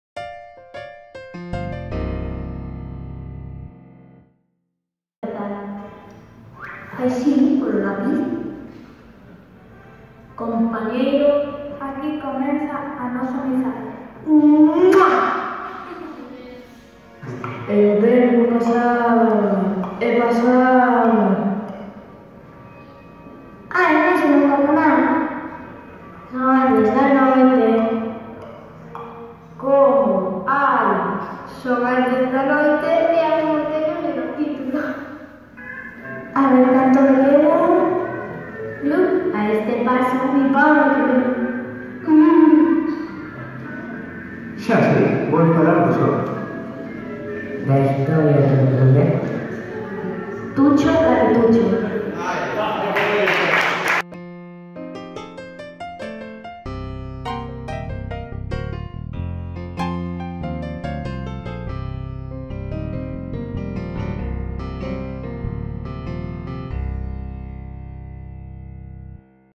Dobraxe na escola
Resultado do traballo realizado no obradoiro de dobraxe na escola que organiou a concellería de educación do Concello de Ourense.